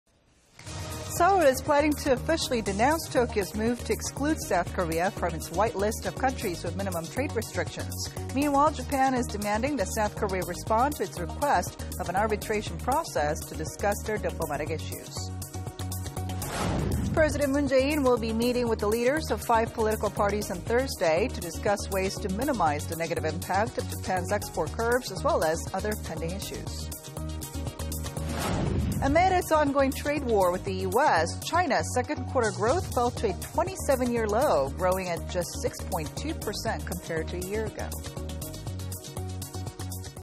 2019.07.16 12:00 NEWS Headlines
Headlines

Seoul is planning to officially denounce Tokyo's move to exclude South Korea from its "white list" of countries with minimum trade restrictions.
Meanwhile, Japan is demanding that South Korea respond to its request... of an arbitration process to discuss their diplomatic issues.

President Moon Jae-in will be meeting with the leaders of five political parties on Thursday... to discuss ways to minimize the negative impact of Japan's export curbs, as well as other pending issues.

Amid its ongoing trade war with the U.S., China's second-quarter growth fell to a 27-year low,... growing at just 6-point-2 percent compared to a year ago.